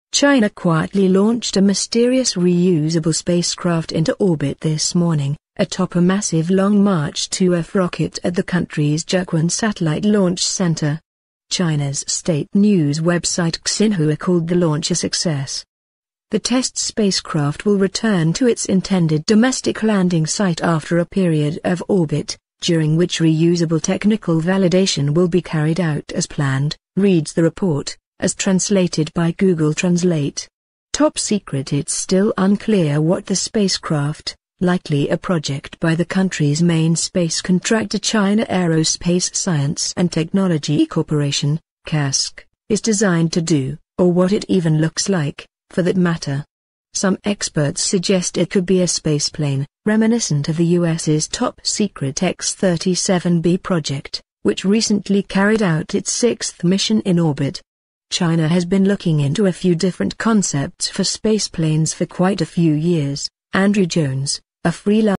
China Just Launched a Mysterious “Reusable Test Spacecraft”
China quietly launched a mysterious reusable spacecraft into orbit this morning, atop a massive Long March 2F rocket at the country’s Jiuquan Satellite Launch Center.

China’s state news website Xinhua called the launch a success. “The test spacecraft will return to its intended domestic landing site after a period of orbit, during which reusable technical validation will be carried out as planned,” reads the report, as translated by Google Translate.

Top Secret
It’s still unclear what the spacecraft, likely a project by the country’s main space contractor China Aerospace Science and Technology Corp. (CASC), is designed to do — or what it even looks like, for that matter.

Some experts suggest it could be a spaceplane, reminiscent of the US’s top secret X-37B project, which recently carried out its sixth mission in orbit.

“China has been looking into a few different concepts for spaceplanes for quite a few years,” Andrew Jones, a freelance reporter specializing in China’s space program, told The Verge.